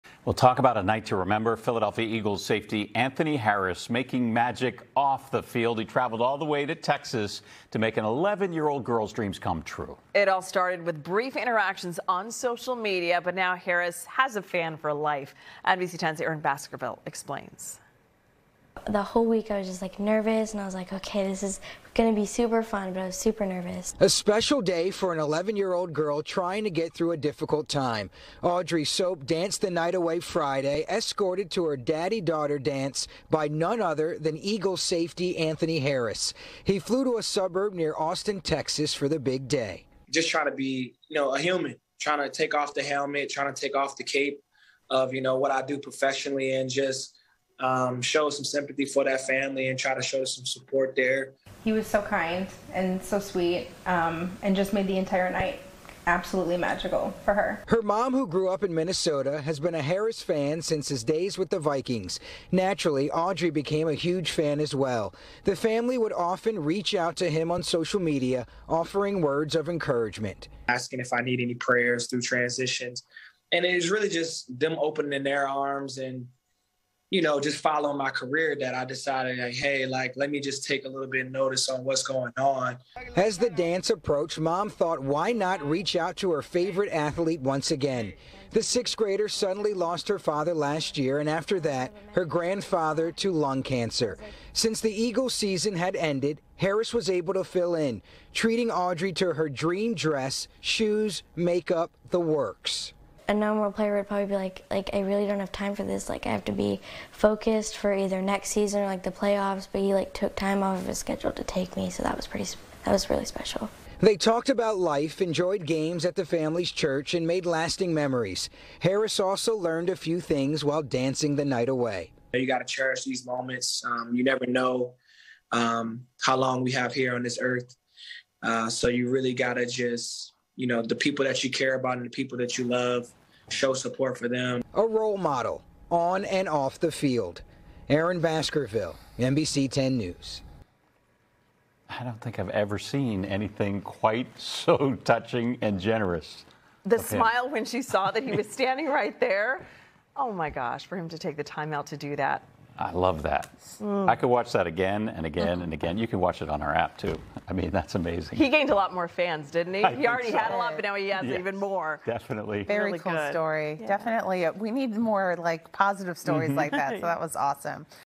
Eagles Player Takes Young Fan to Daddy-Daughter Dance After Her Father's Death
Audrey Soape and her family have been fans of Eagles safety Anthony Harris since he played for the Vikings. After Audrey’s father and grandfather died, Harris traveled all the way to Texas to take her to a daddy-daughter dance. The family spoke with NBC10’s Aaron Baskerville about Harris’ heartwarming gesture.